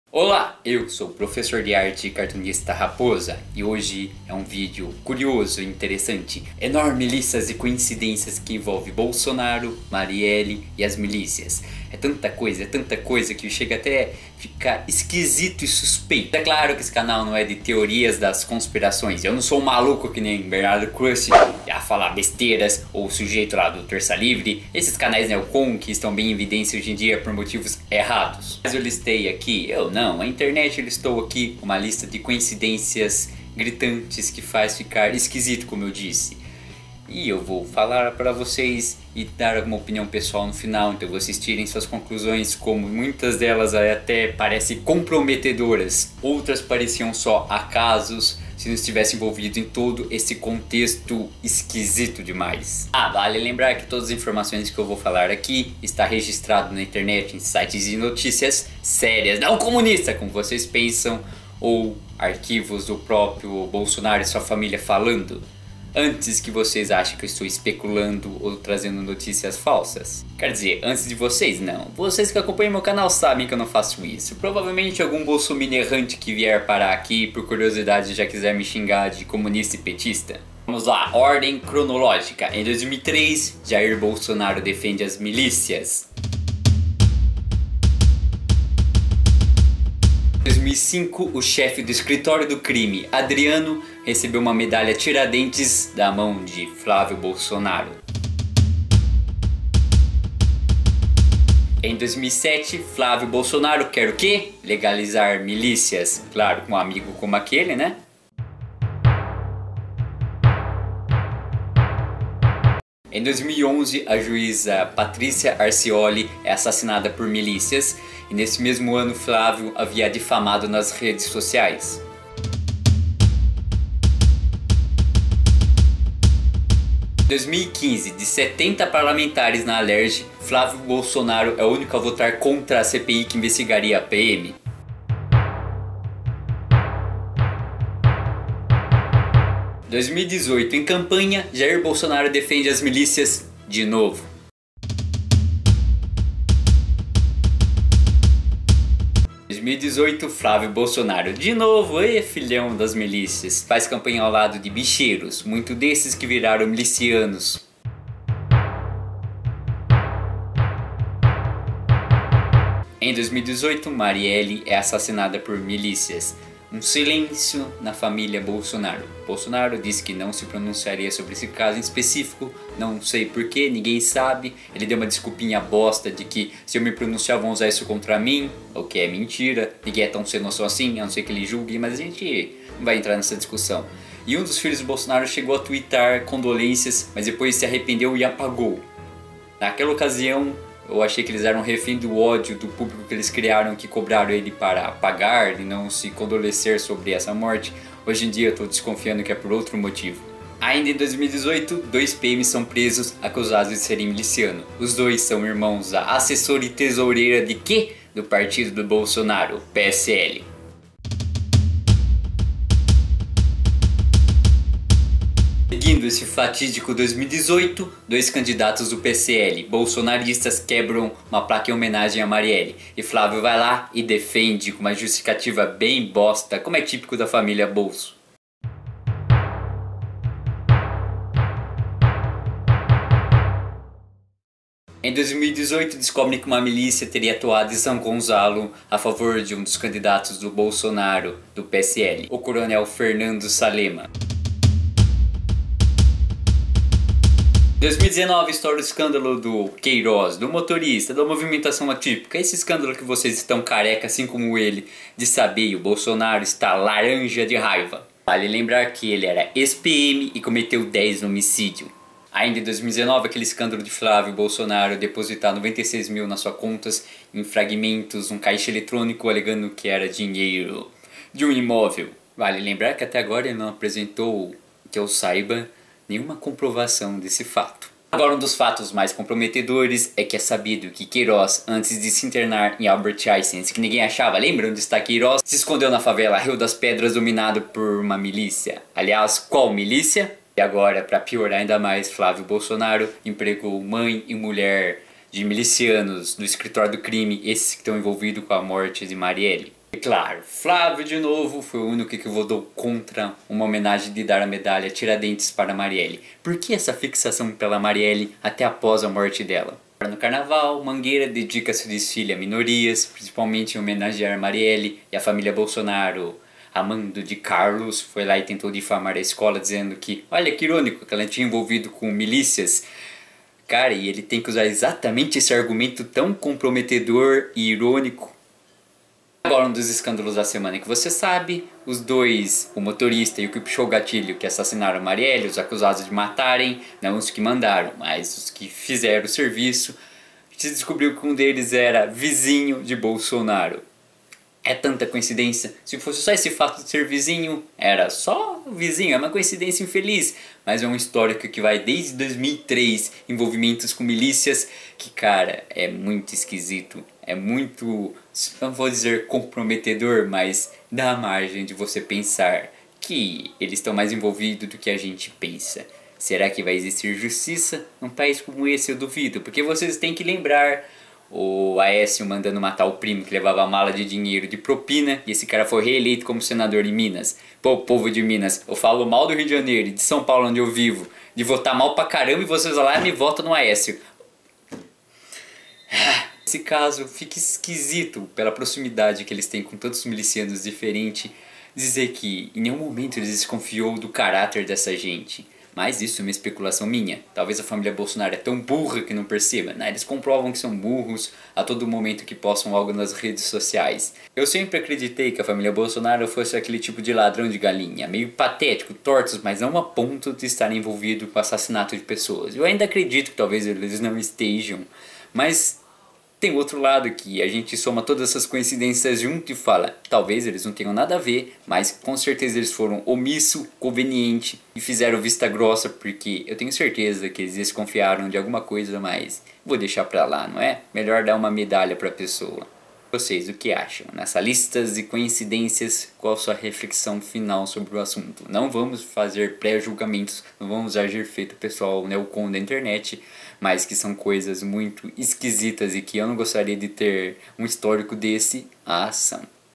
As "COINCIDÊNCIAS" de BOLSONARO no caso MARIELLE
Bolsonaro tem ligação com milícias desde muito tempo. Milicias essa envolvida no caso Marielle, a familia já mostrou desprezo publico a vereadora.

No vídeo de hoje eu reuni essas coincidências!